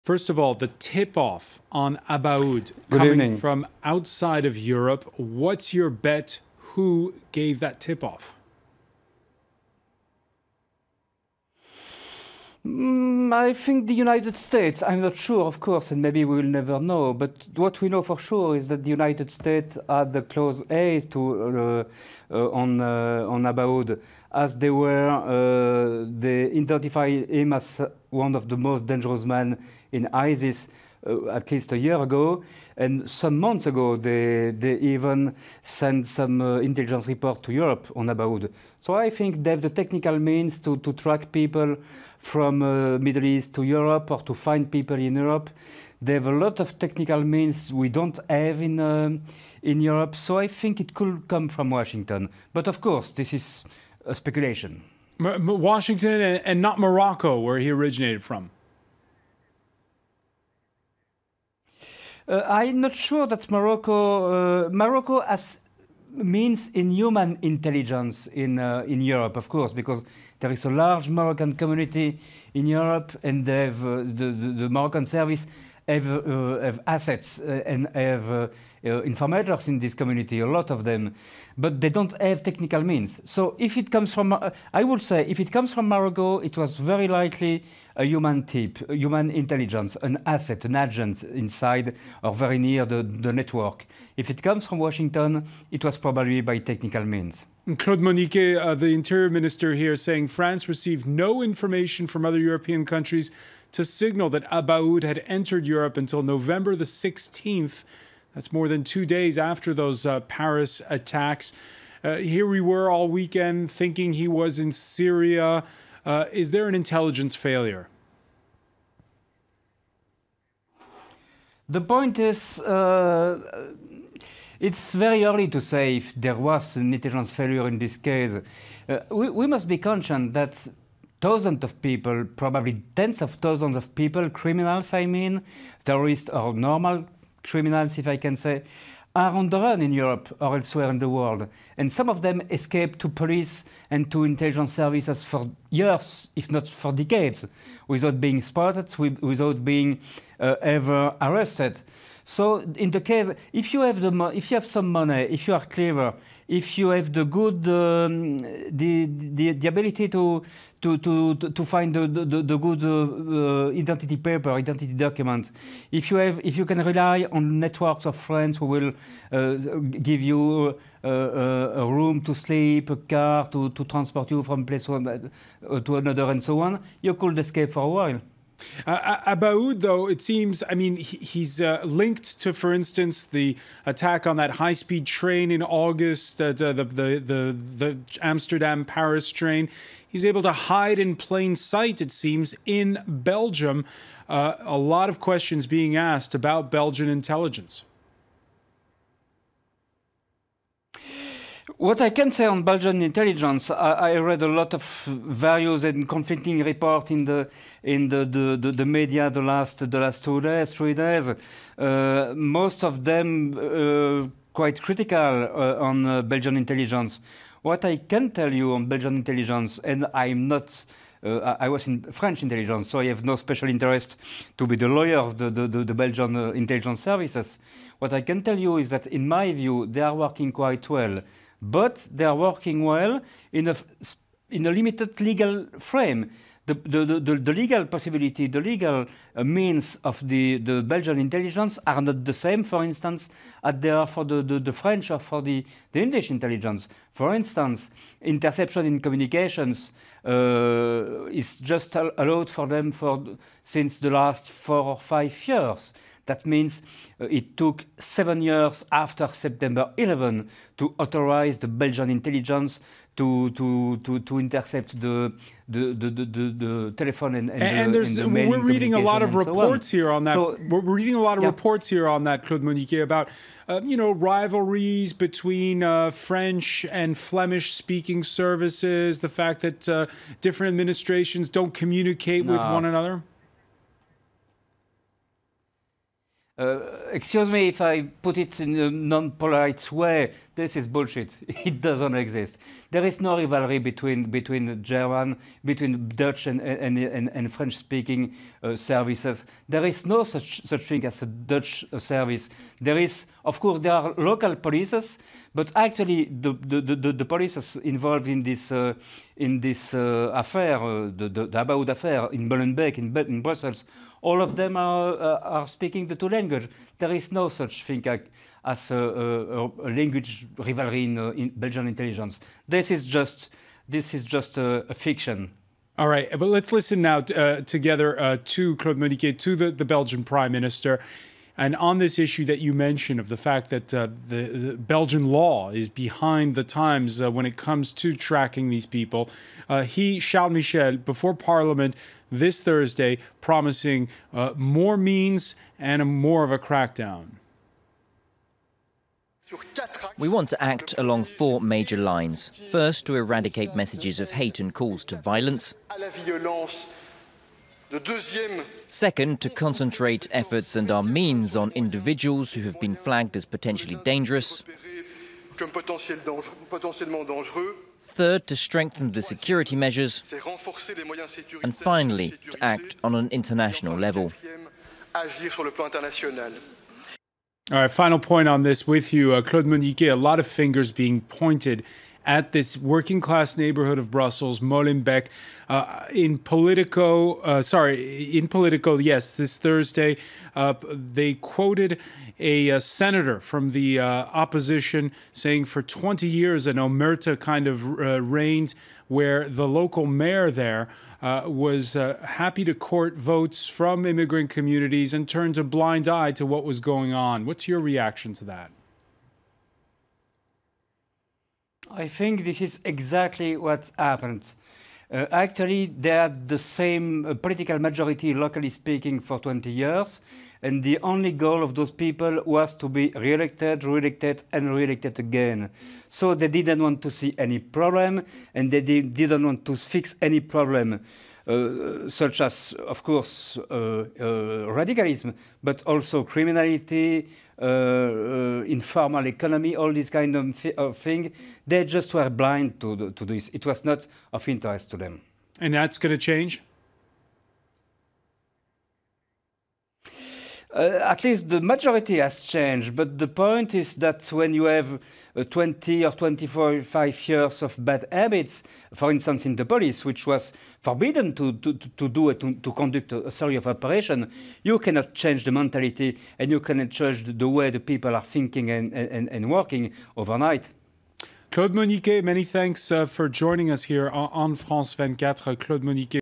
Paris Attacks: Claude Moniquet slams Molenbeek's mayor, "blind on radicalism and criminality"
European Strategic Intelligence and Security Center researcher Claude Moniquet reacts to the Paris Attacks and the ongoing manhunt on the prime suspects, especially in Belgium and in the Brussels' neighbourhood of Molenbeek. He slams the mayor of this neigbourhood, saying that "he just wanted to be reelected again, and did not want to fix